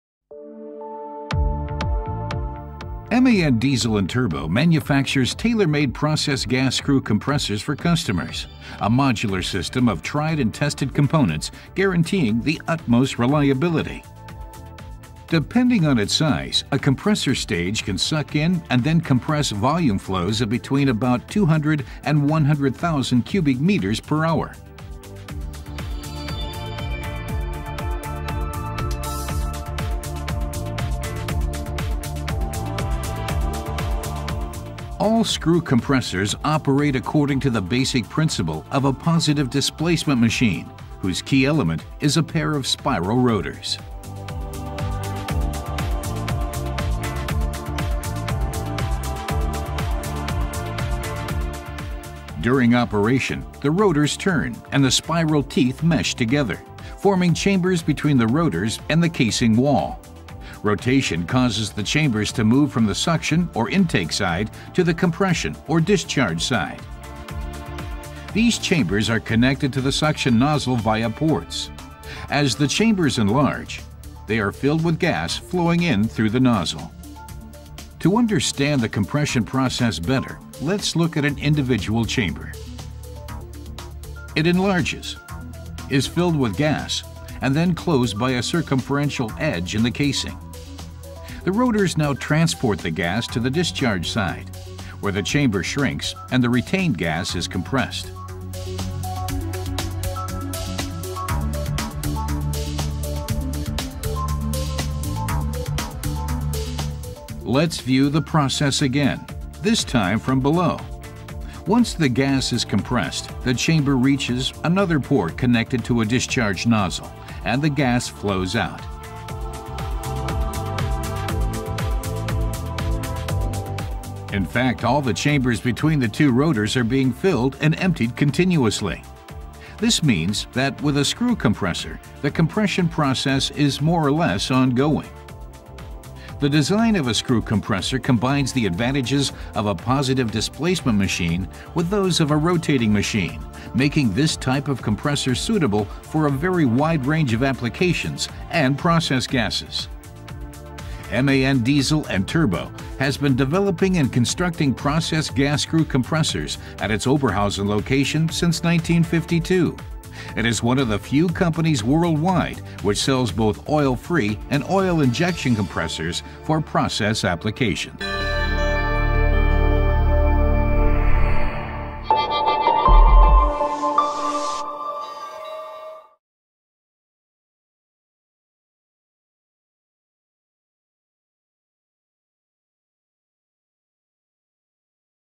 Screw Compressor How that works
The working principle of screw compressor.
A video credit to man and turbo.